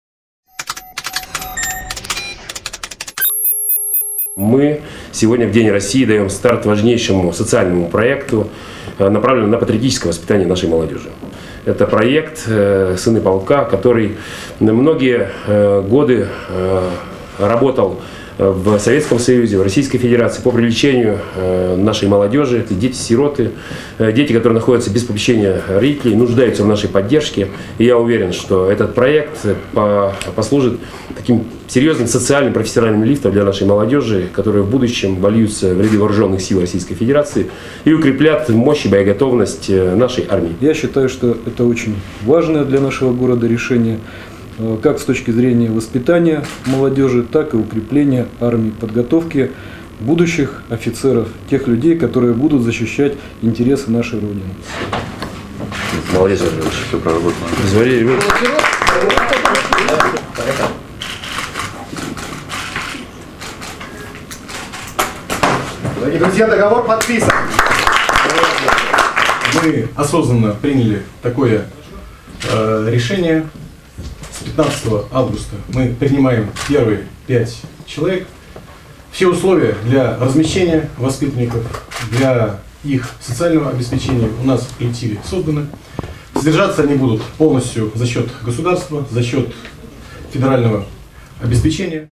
12.06.2016 Подписание соглашения по "Сынам полка"
1 сентября пять подростков, оставшихся без родителей, станут курсантами череповецкого военного училища.